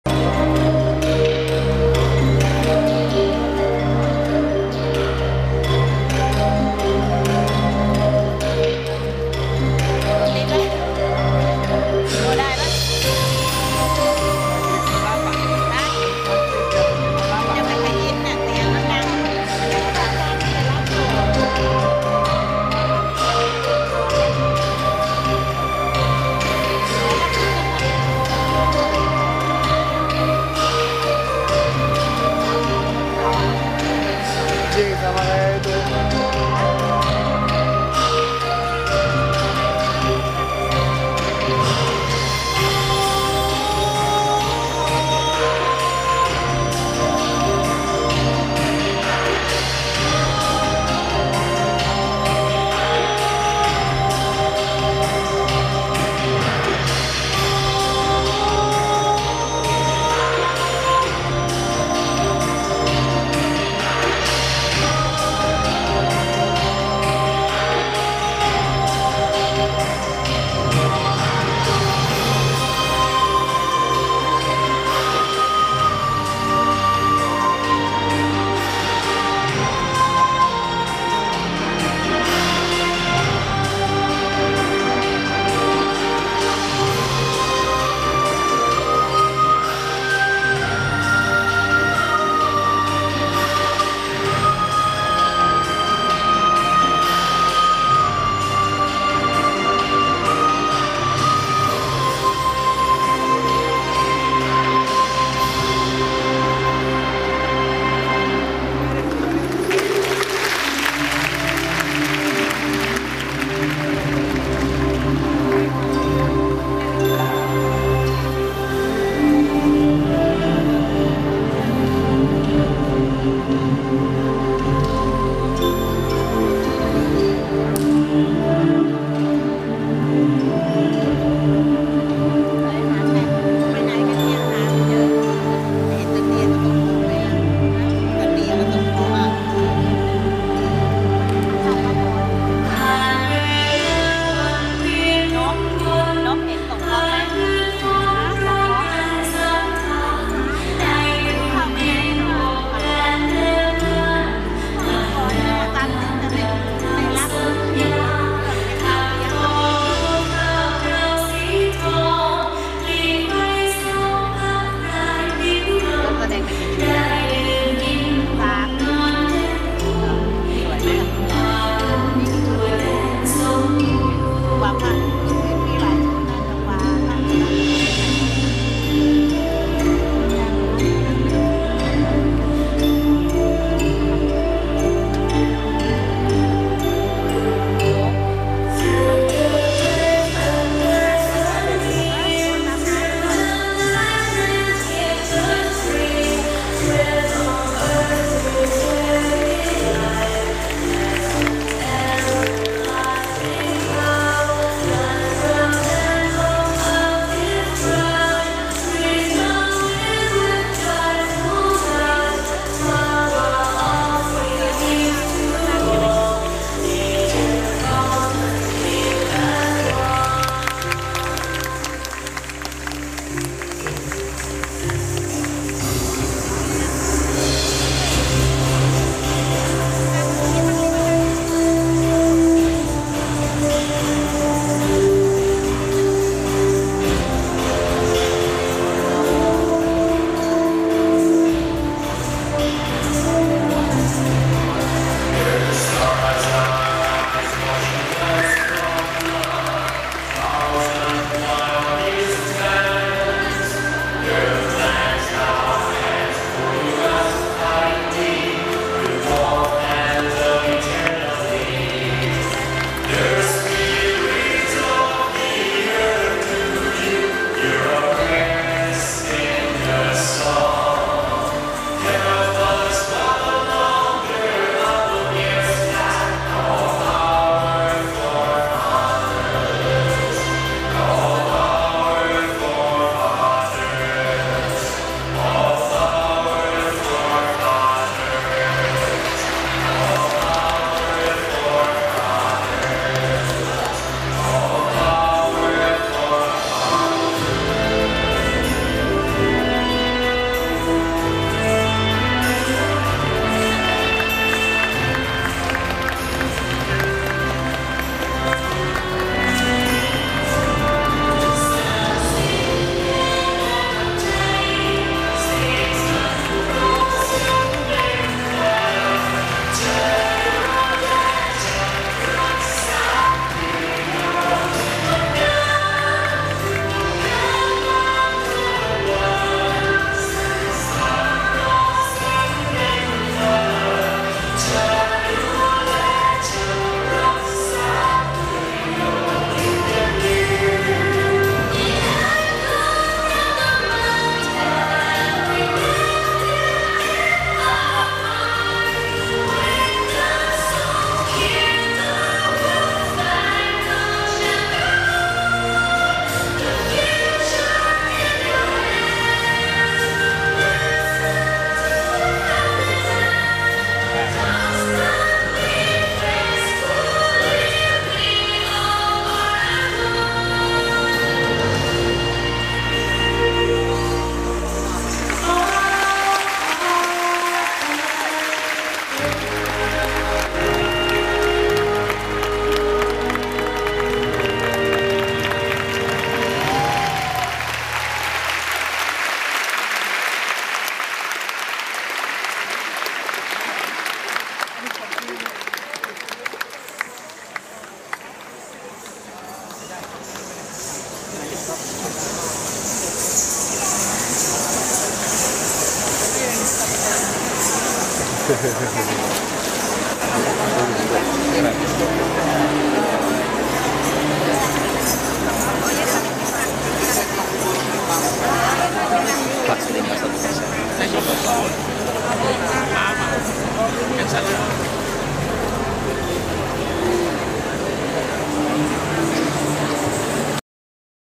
Opening Ceremonial in Thailand Cultural Center, Bangkok.
It performed with many countries. I'm sorry if not in HD because the actual size is very big.